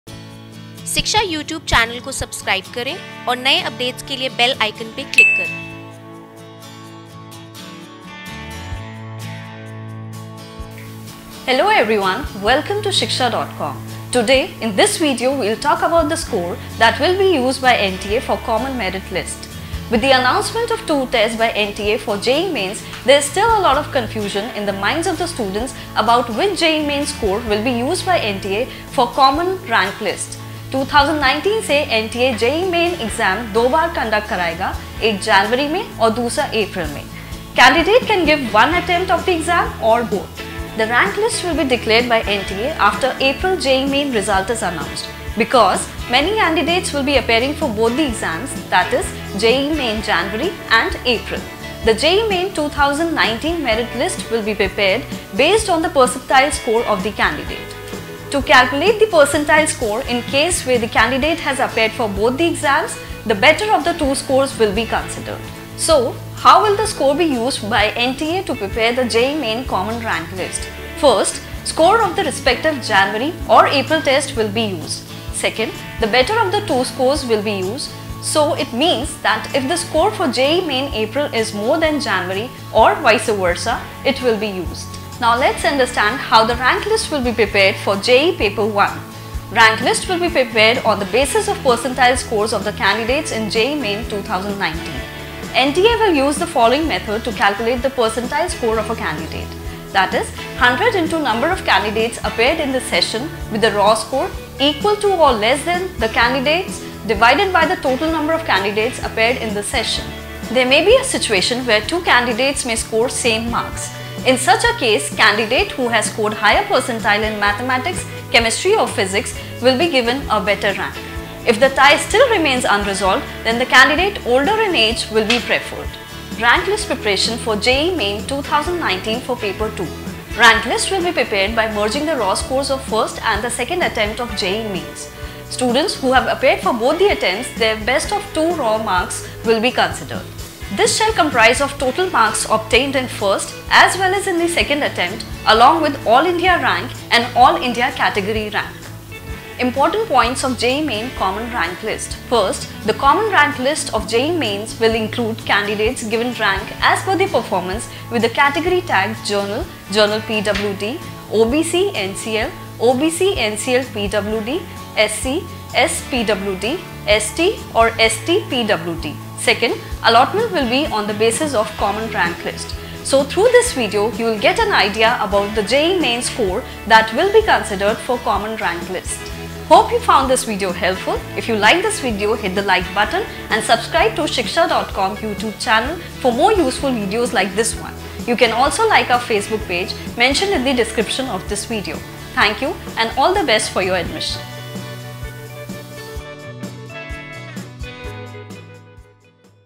JEE Mains- Rank Calculation by NTA
Today in this video we’ll talk about the score that will be used by NTA for Common Merit List
With the announcement of two tests by NTA for JEE Mains there is still a lot of confusion in the minds of students about which JEE Main score will be used by NTA for the common merit rank list.The rank list will be declared by NTA after April JEE Mains result is announced because many candidates will be appearing for both the exams – JEE Main January and April. The JEE Main 2019 rank/ merit list will be prepared based on the percentile score of the candidate.
To calculate the percentile score, in the case where the candidate has appeared for both the exam, the better of the two scores will be considered.